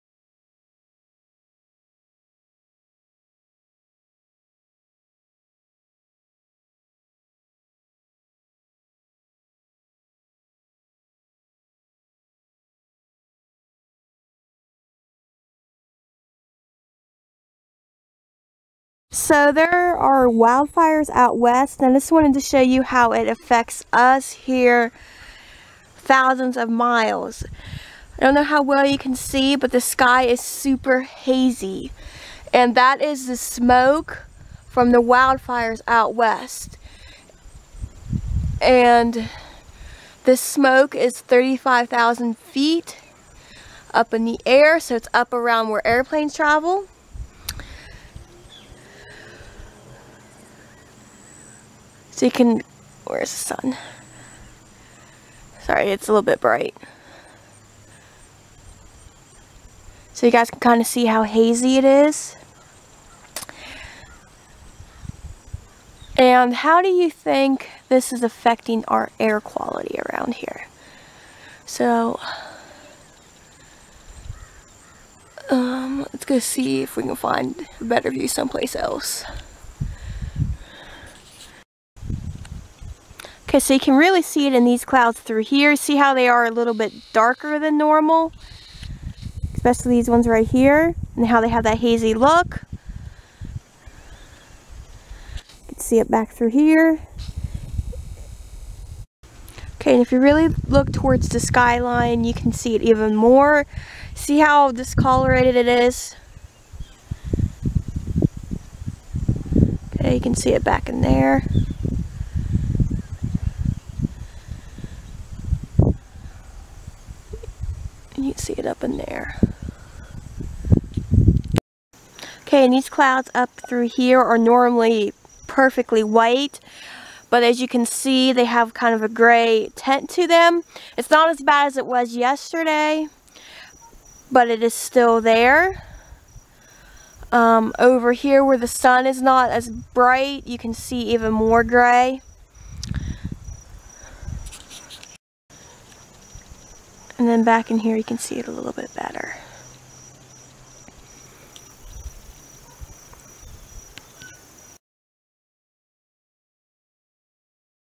Earth Science: Why Is the Sky Gray?
Description:
Today I am bringing you a little mini Earth Science lesson! In September 2020, skies across the US turned hazy as a result of wildfires on the West Coast. Take a look at the skies and see if you can find anything different than normal.

Subject: Earth Science
Topic: Wildfires and Natural Disasters
 Grades: 4th-8th